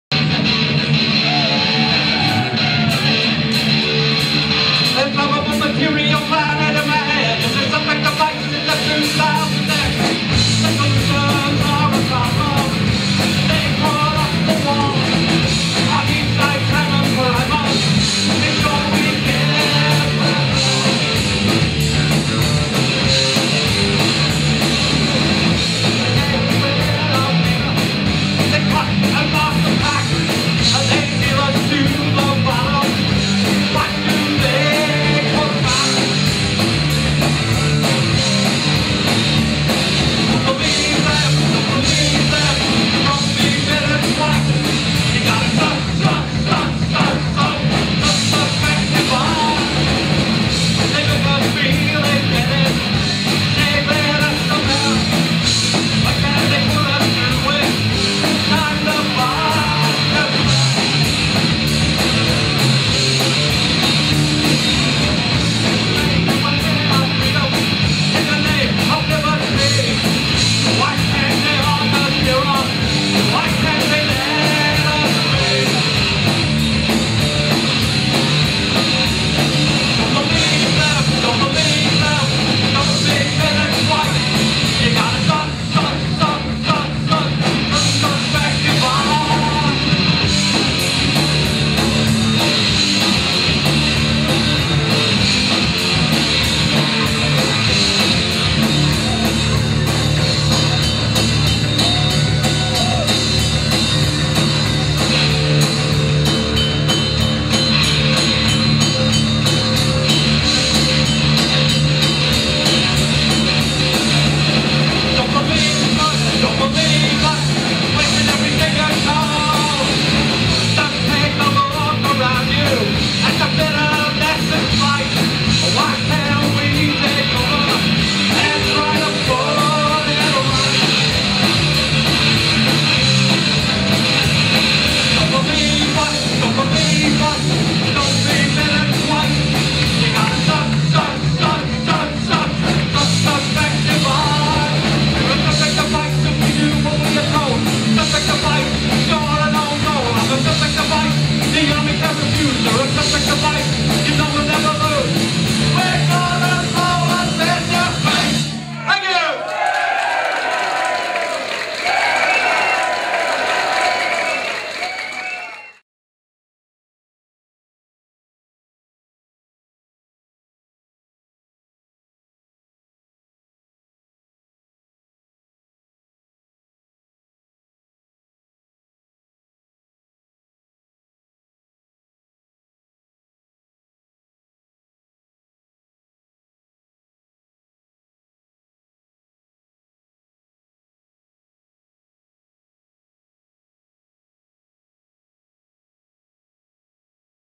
XSLF Strummercamp Festival 2014
XSLF  at Strummercamp Festival 2014  probably one of the best independent and not-for-profit music festivals.
Jelly Universe Productions  will be playing out the footage of all the bands filmed in a live youtube event to promote Strummercamp 10 in the next few weeks.